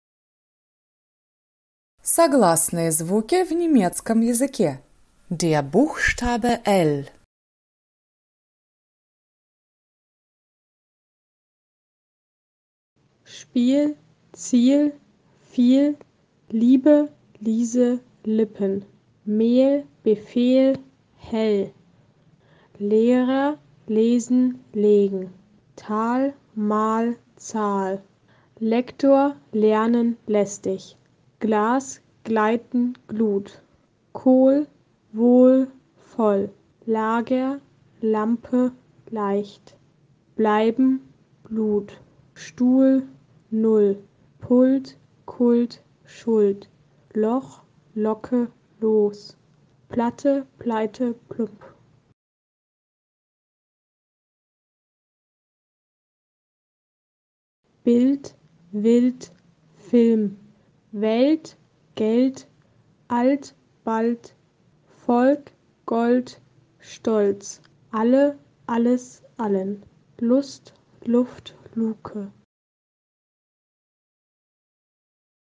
Aussprache Konsonanten T.4 - L (Deutsch A1)
Немецкое L (Эль)   -  нечто среднее между мягким ль [л'] и твердым эл[л], но ближе к мягкому варианту.
Важно,что  немецкое  L  ( как все немецкие согласные)  произносится  длительнее и напряжённее, чем в русском.

Задание:  Посмотрите видеоурок. Повторите  за носителем языка примеры несколько раз. Запишите себя на диктофон. Сравните. В случае необходимости – переслушайте примеры в видео и перезапишите, пока не начнет получаться.
Отметьте плюсиком под постом у кого получилось  и  напишите, сколько раз пришлось переслушивать и перезаписывать.

Ролик представляет собой визуализацию отрывков
из учебника Андрющенко Т.Я. ("Немецкий язык. Начальный этап")

Наш сериал " Жизнь с акцентом"
про жизнь студентов журфака МГУ  (про любовь!).